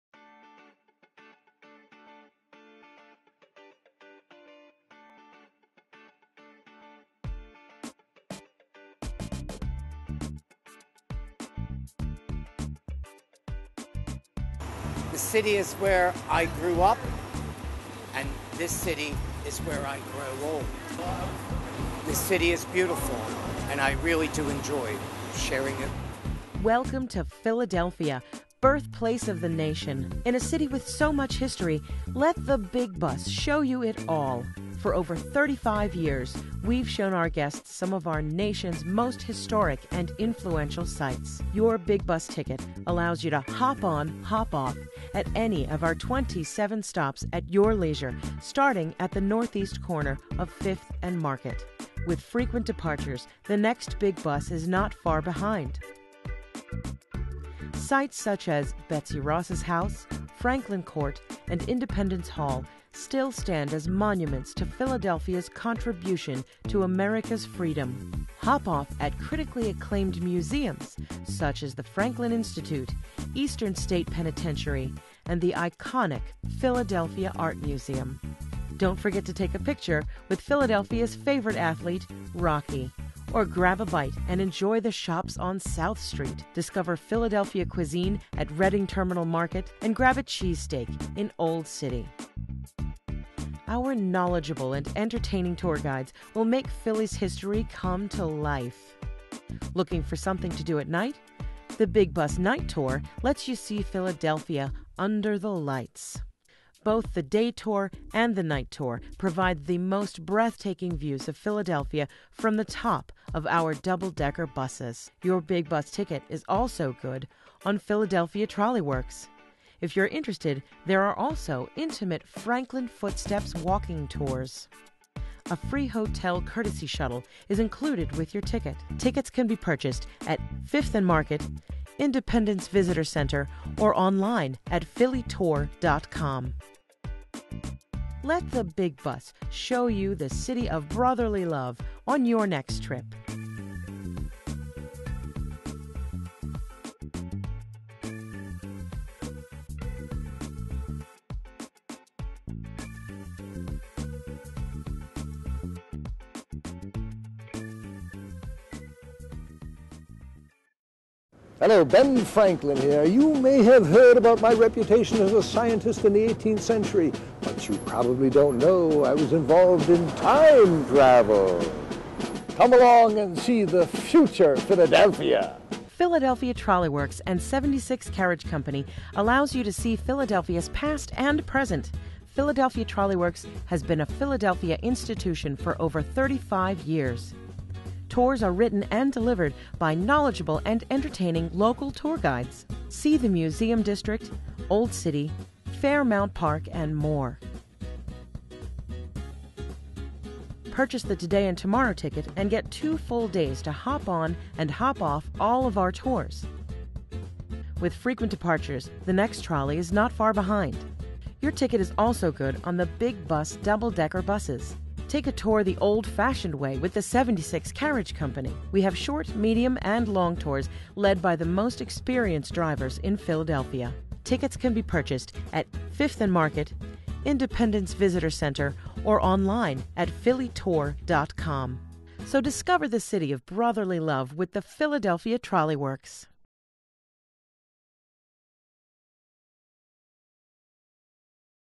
Philadelphia Trolley Works and Big Bus Company
Experience Philadelphia from the Top. The Top of a Modern Double Decker Bus! See the sights of the city on one of our Open-Top tours that offer spectacular views and entertaining stories from our fully-trained Philly friendly guides. Professional Tour Guides bring old and new Philadelphia to life on our 90+ minute fully-narrated sightseeing "City Tour".

Your Carriage Awaits! Sit back, relax and experience Philadelphia's history and other popular attractions aboard one of our stylish coaches. Journey back in time on an enchanting carriage ride through the tree-lined streets of Philadelphia's Independence National Historic Park, Society Hill and Old City. Your personal Guide will whisk you away on a leisurely ride through historic Philadelphia.